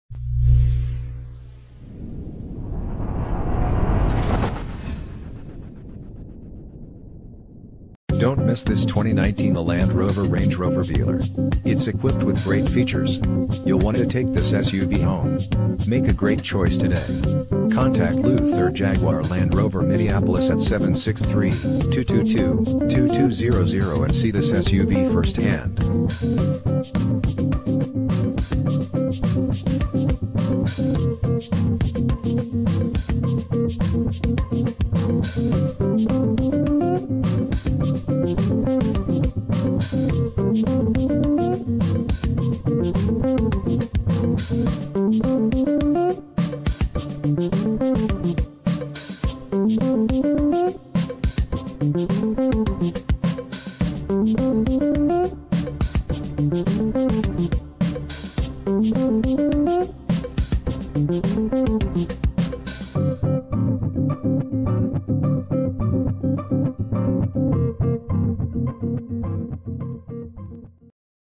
2019 Land Rover Range Rover Velar Minneapolis, Golden Valley, St Paul, MN P7116
2019 Land Rover Range Rover Velar R-Dynamic SE

Don't miss this 2019 Land Rover Range Rover Velar. It's equipped with great features. You'll want to take this suv home. Make a great choice today.